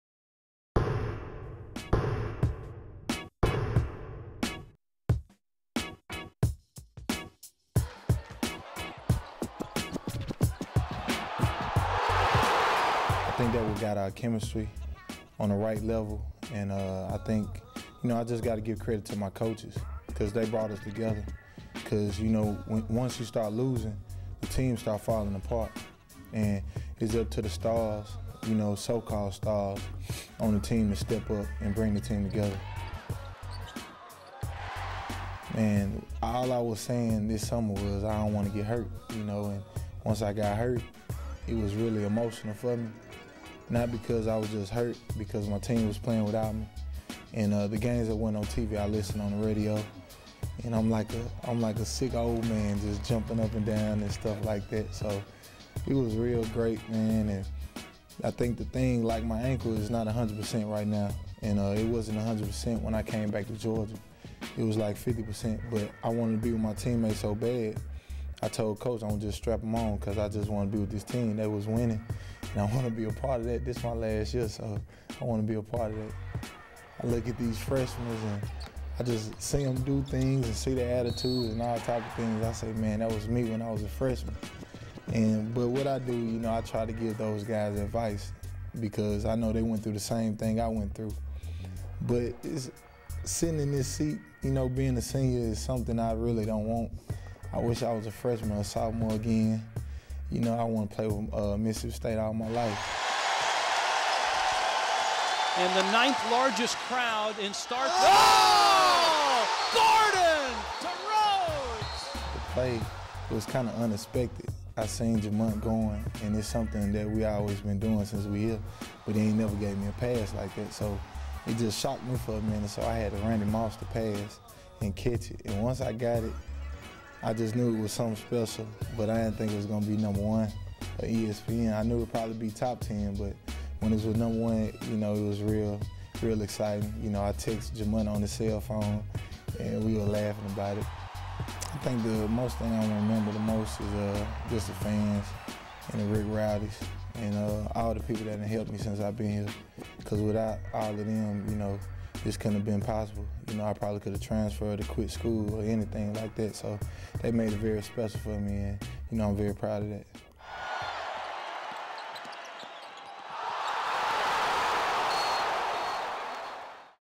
Player Profile
Off-the-court with Charles Rhodes is a segment from Mississippi State basketball with Coach Stansbury.

Editor // Tanner Cade
Graphics // C.J. LeMaster
Camera // Bryan Champlin